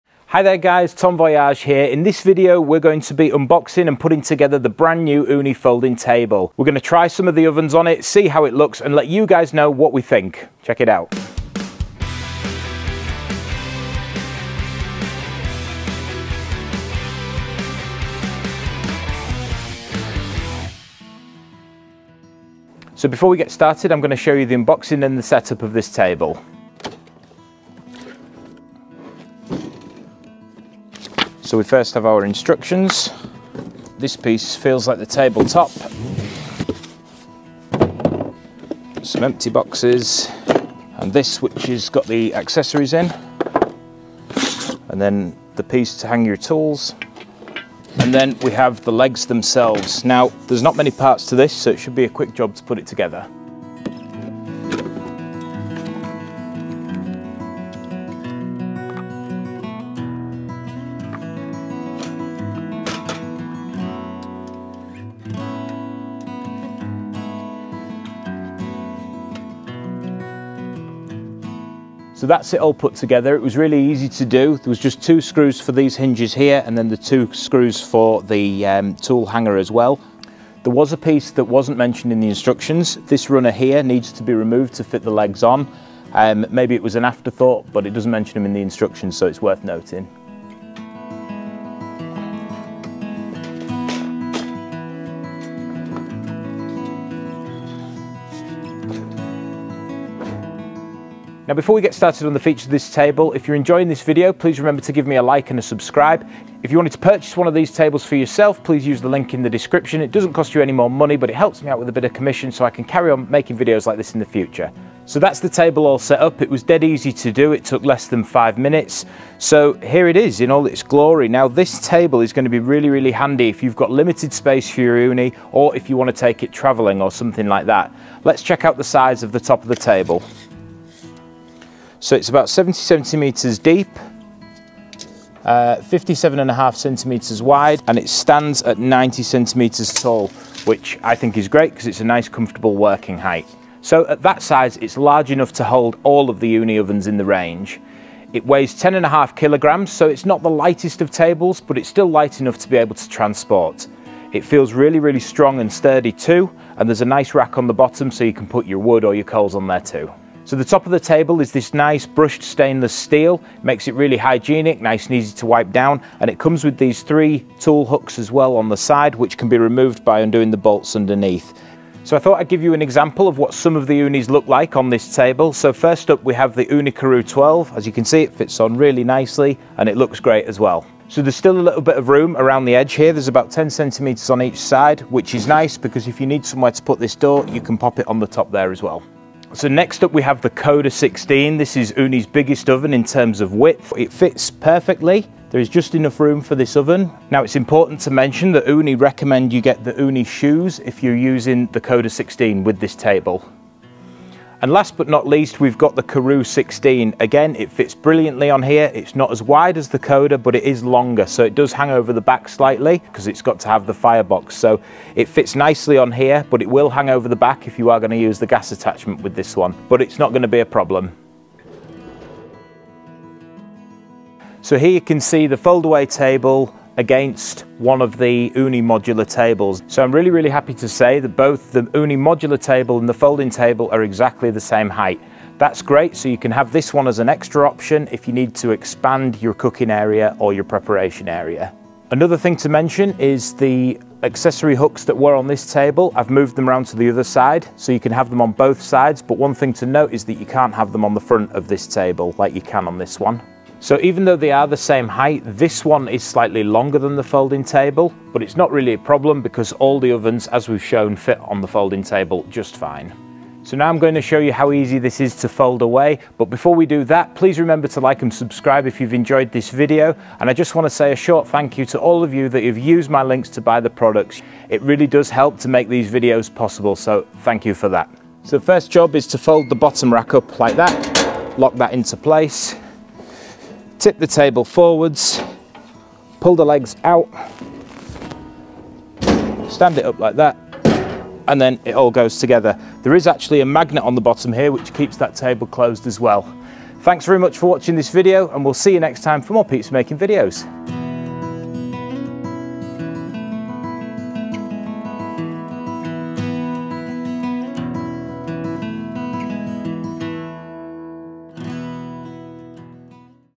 Ooni Folding Table In Depth Review & Pizza Oven Comparison
This is a video review of the Ooni Folding Table, and test comparison of how each Ooni oven fits on the table. This is a great product ideal for those with limited space or wanting to take their pizza oven travelling.

⬇️PRODUCTS/DOUGH USED IN THIS VIDEO ⬇️
🌡 Erick Hill Laser Thermometer